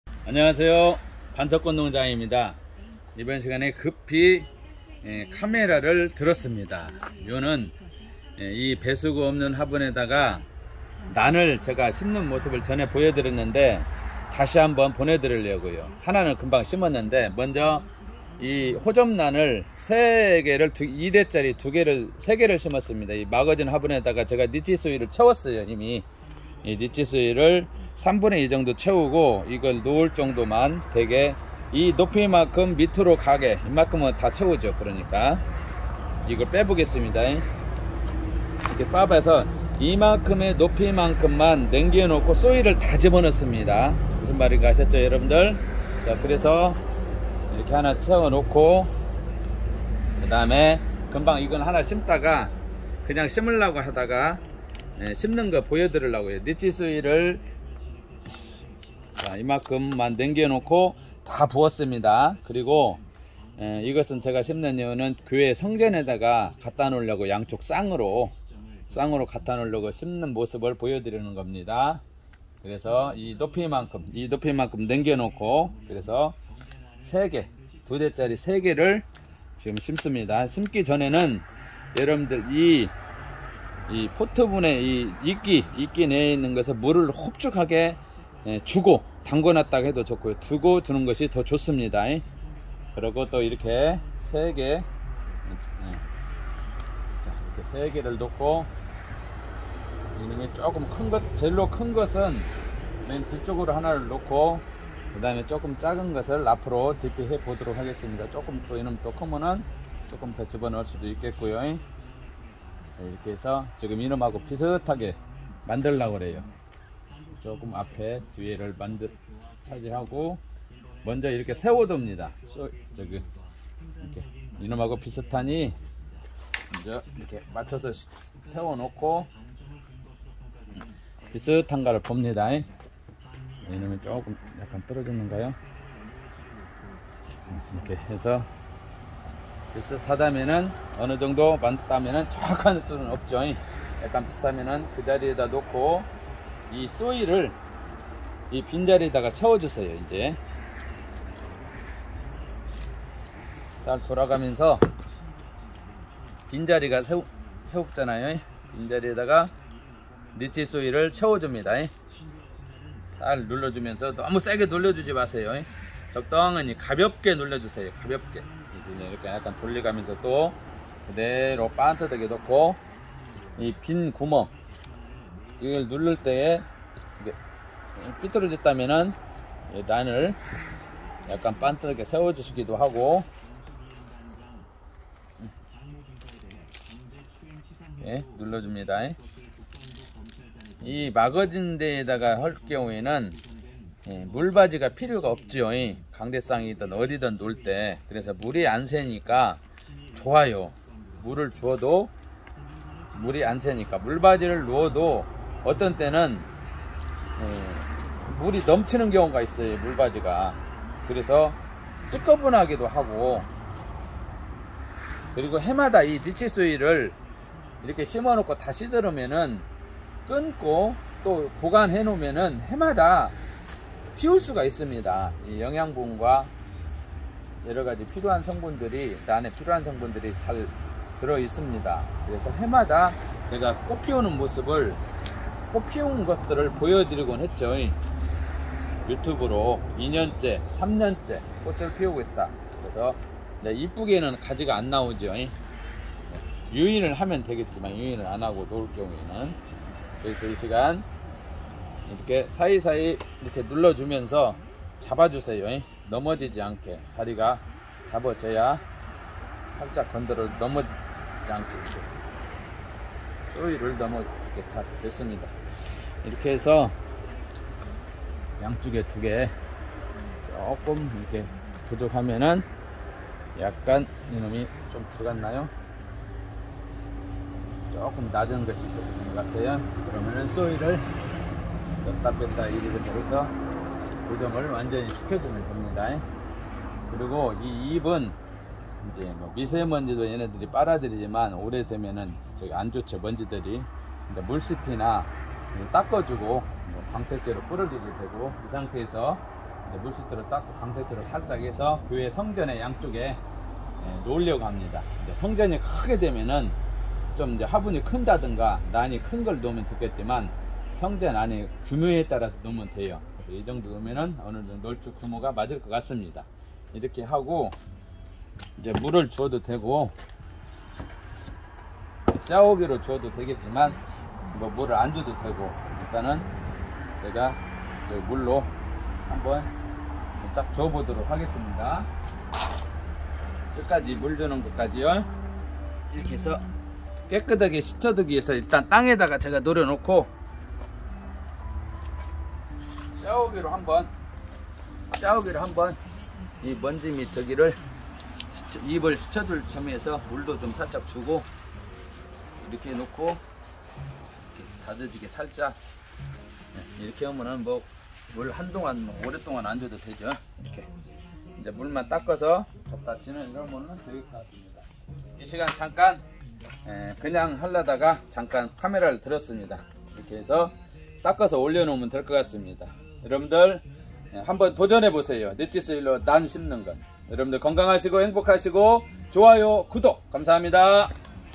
호접란 리치쏘일로 분갈이합니다
리치쏘일주문은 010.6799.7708로 이름 주소 폰번호 수량주세요 6리터1만원 20리터2만8천원 50리터4만8천원 입니다